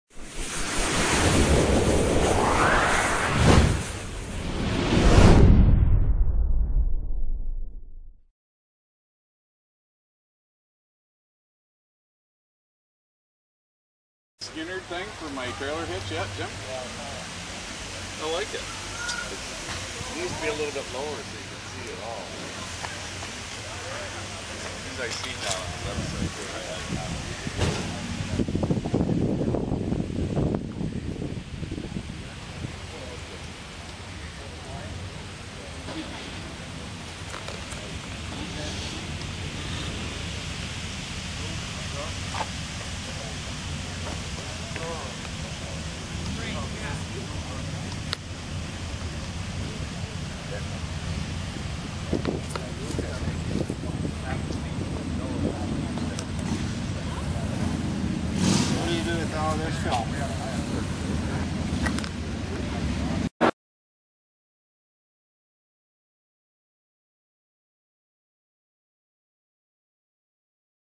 1967 Chevy Chevelle SS
Coverage from the Griffin Autotek Car show in Sarnia, Ontario, July 28, 2012